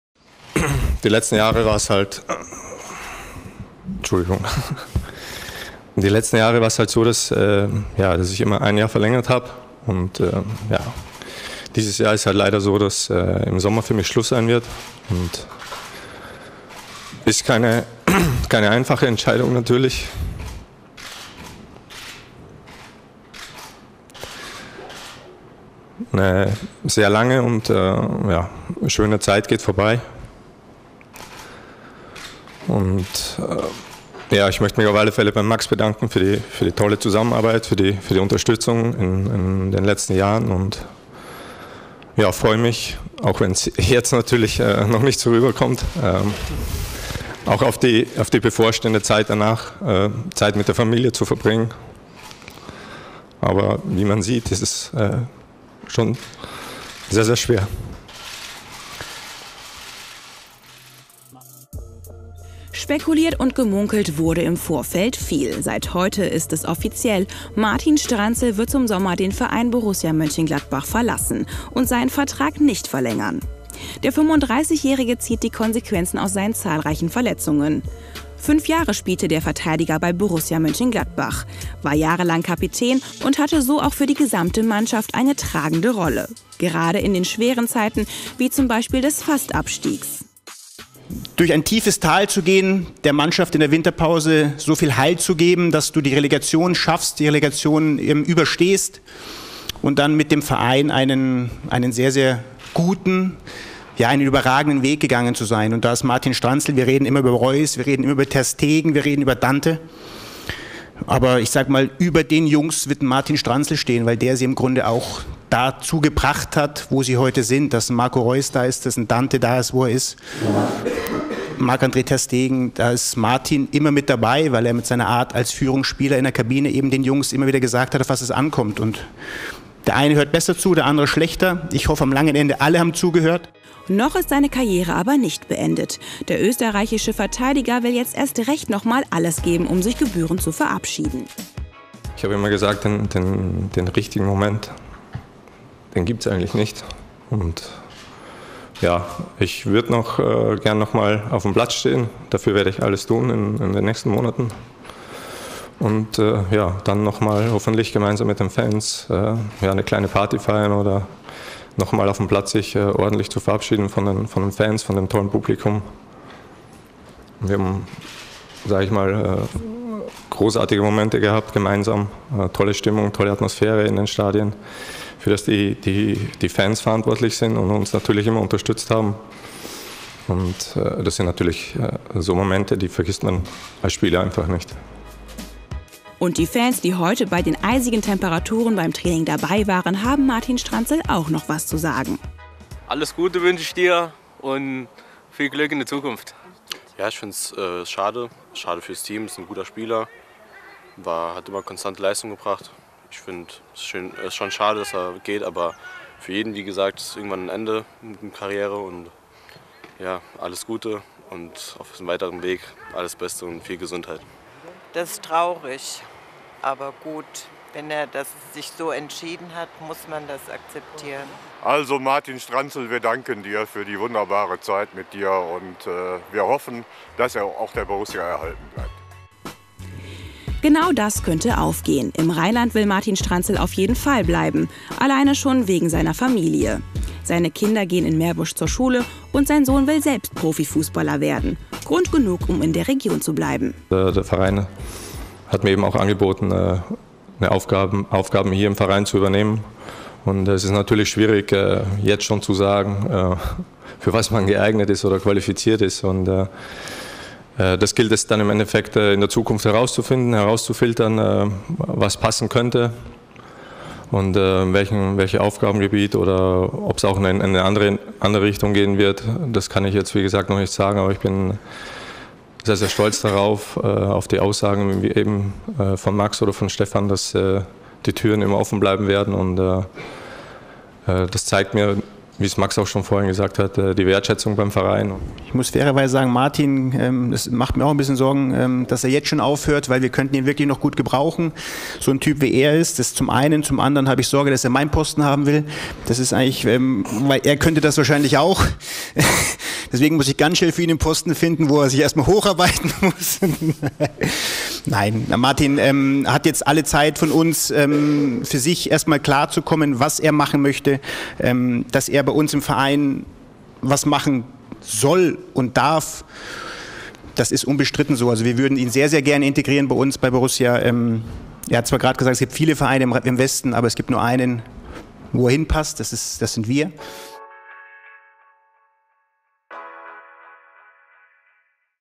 Martin Stranzl beendet seine Karriere
Wir waren bei der Pressekonferenz dabei. Ein paar Tränchen sind auch geflossen.
Wir sagen DANKE Martin Stranzl.
Postet Eure Zukunftswünsche für Martin gerne in die Kommentare.